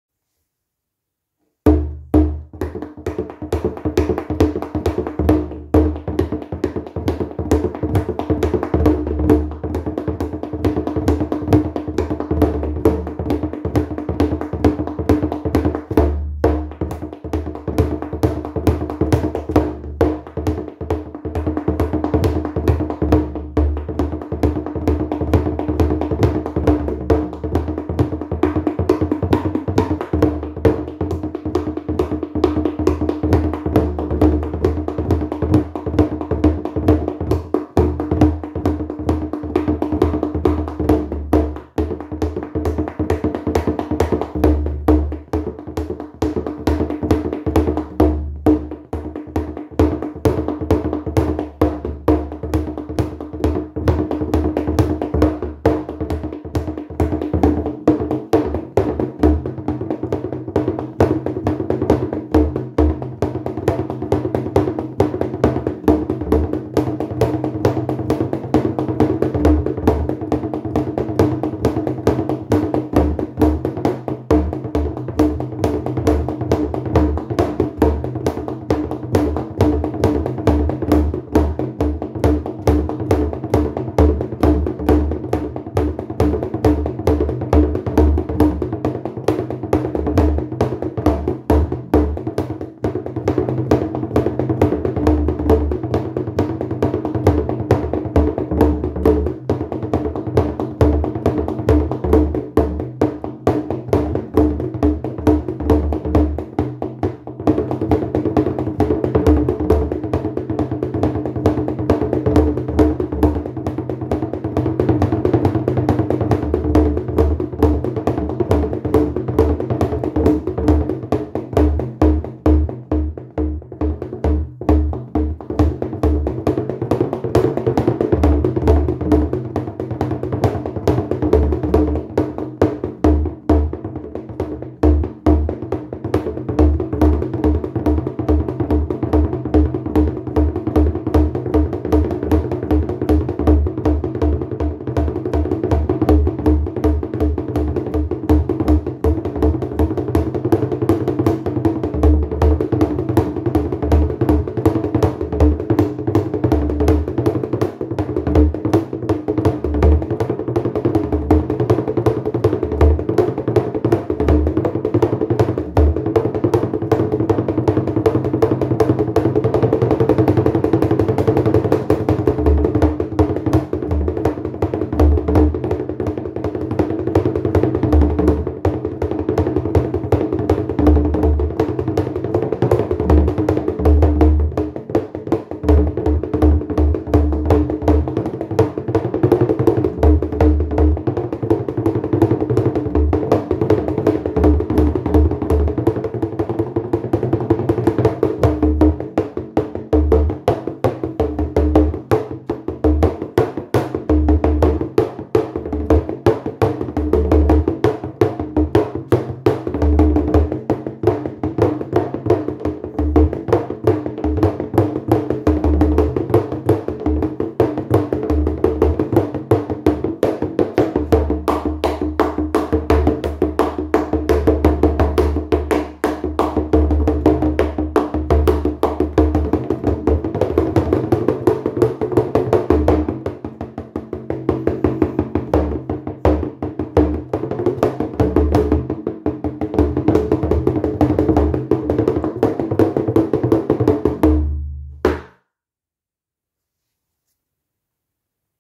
John Joe Tommy Hybrid
This grip works like a hybrid between a John Joe Kelly matched grip style and Tommy Hayes's grip. The stick us striking the skin at a perpendicular angle so you can get loads if volume.